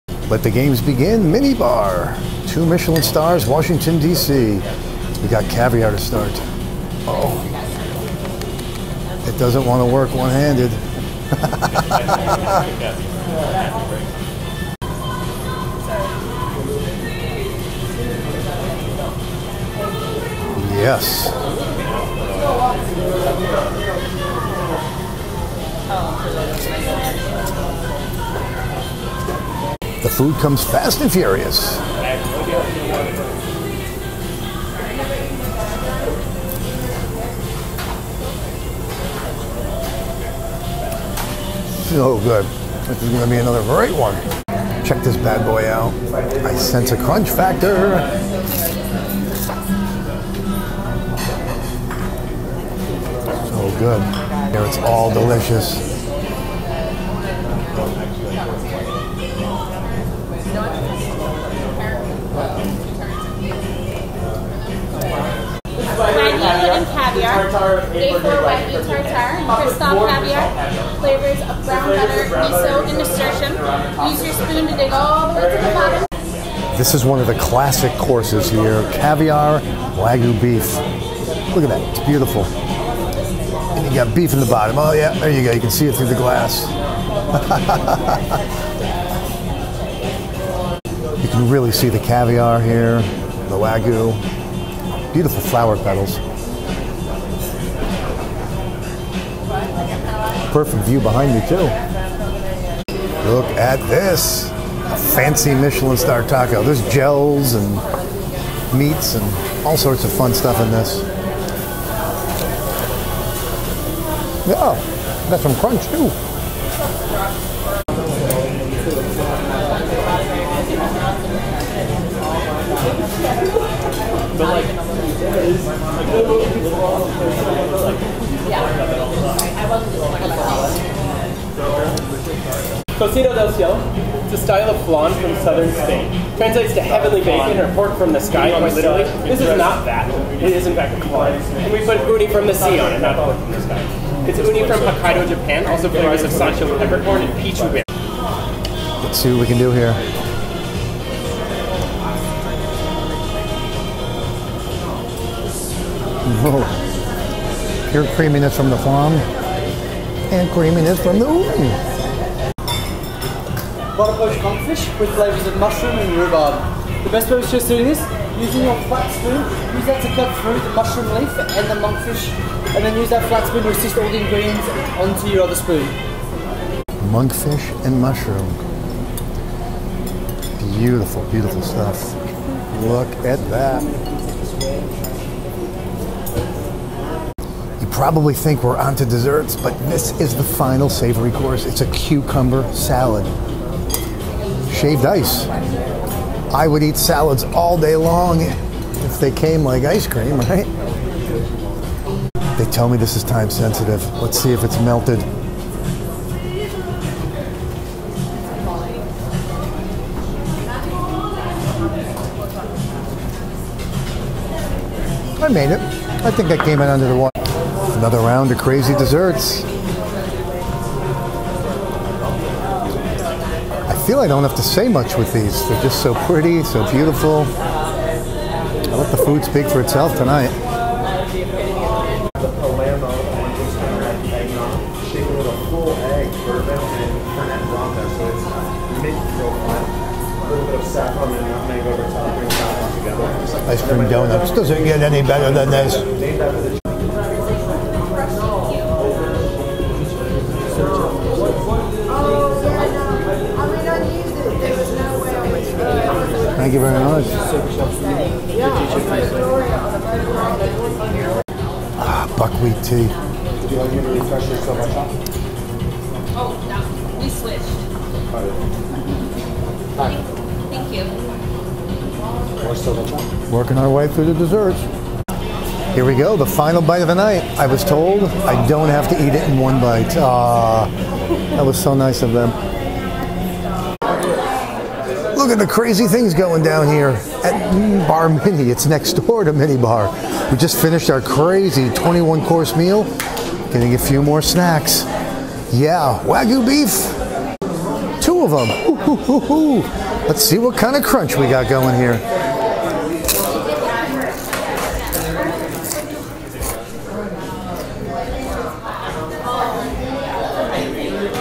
Jose Andres 2 Michelin Star Culinary Temple Minibar in Washington DC
Restaurants don't get much better or fun than this. Two Michelin Stars of magical culinary wizard.